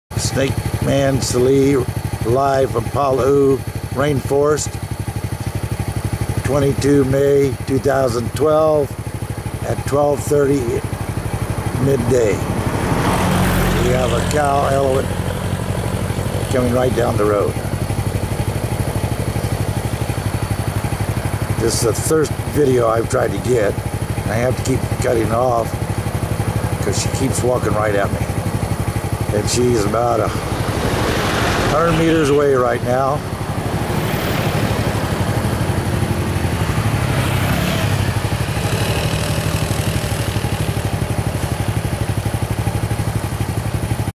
ELEPHANT, PALA U RAIN FOREST, THAILAND, MAY 22, 2012
HI FOLKS THIS IS THE SAME ELEPHANT AND I HAVE TO KEEP MOVING. ENJOY, SNAKE MAN SALLEE